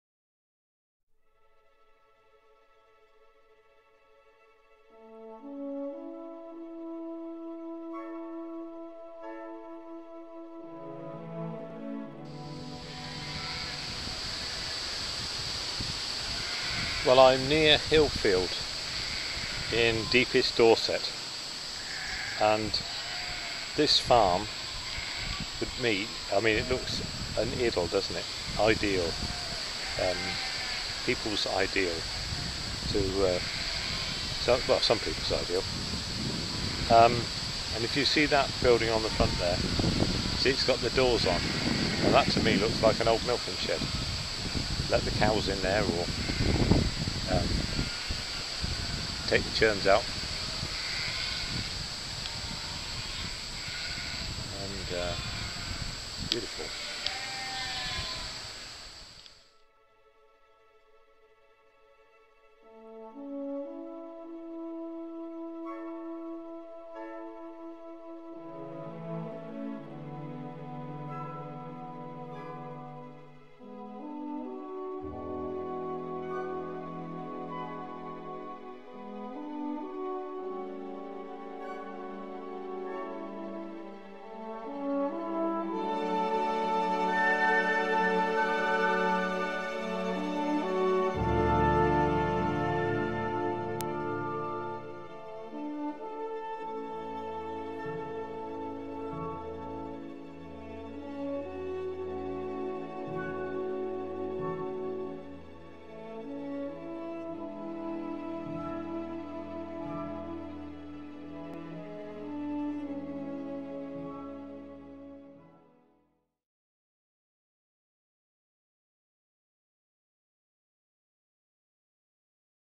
Hermitage ~ Hilfield, Dorset, June Views
These are views within four miles of where I live; we live in a holiday location, so I may as well make the most of it.  Dorset is green, leafy and open, a softer landscape than Devon or Cornwall and dryer than Somerset.
Filmed using Vivitar DVR 830XHD also using the camera facility.